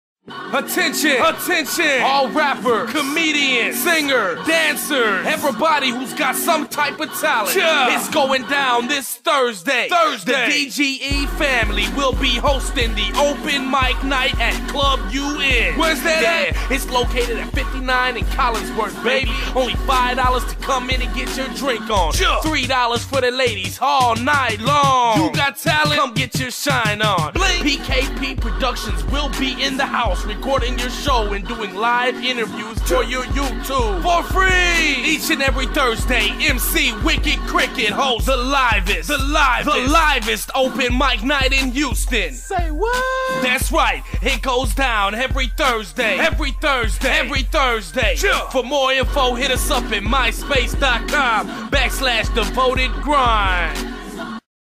Open Mic at Club UN Every Thursday Night In Houston Tx (5th Ward)
ITs going down next thursday
Night @ the UN!! 59 @ collingsworth baby!! $5 to come in and get Ur shine on $3 for the ladies all night long!!! rappers,comedians,singers
all invited!!! COME GET YOUR SHINE ON!! PKP will be in the house
recording your show and doing live interviews for your youtube channel..
... its going down next Thursday.. for more info hit me up..